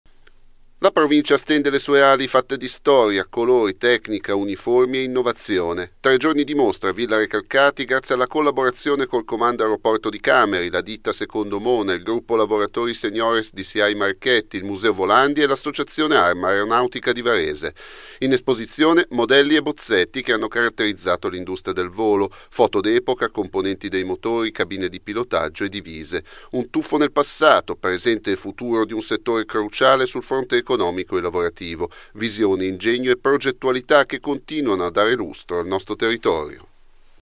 Varese: 100 anni di Aeronautica in mostra a Villa Recalcati
La Provincia stende le sue ali, fatte di Storia, colori, tecnica e innovazione.
Tre giorni di mostra (17-19 Maggio) a Villa Recalcati, grazie alla Collaborazione con il Comando Aeroporto Cameri, la ditta Secondo Mona, Gruppo lavoratori Seniores di SIAI Marchetti, Museo Volandia e A.A.A Varese
In esposizione, modelli e bozzetti, foto d’epoca, componenti dei motori, cabine di pilotaggio e divise.